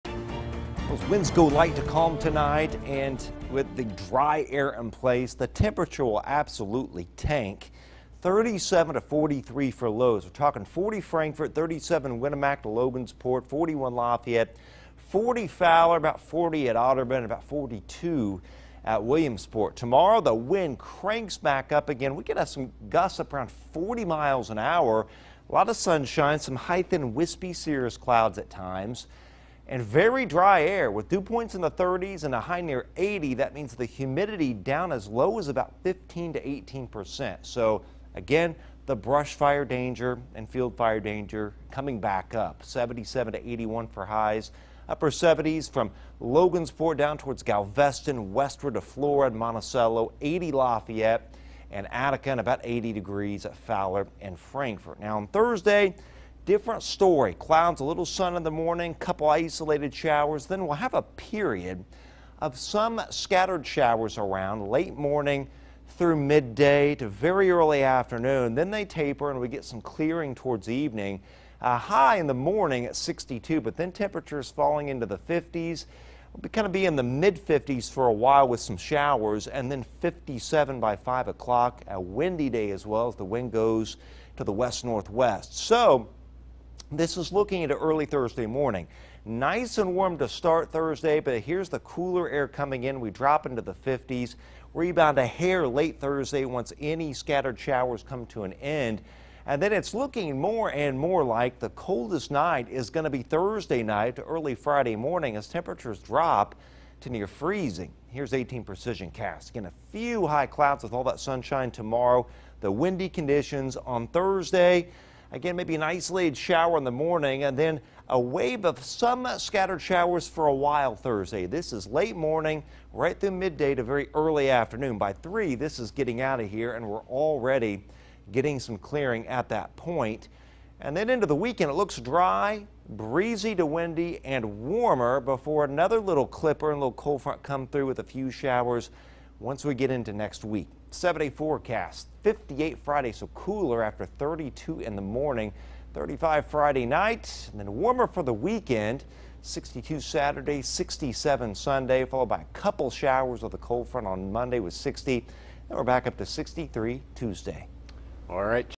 Precision 18 Forecast (10/13/20): Dry, warm & windy, followed by some showers & a cool-down.
Dry, warm & windy, followed by some showers & a cool-down.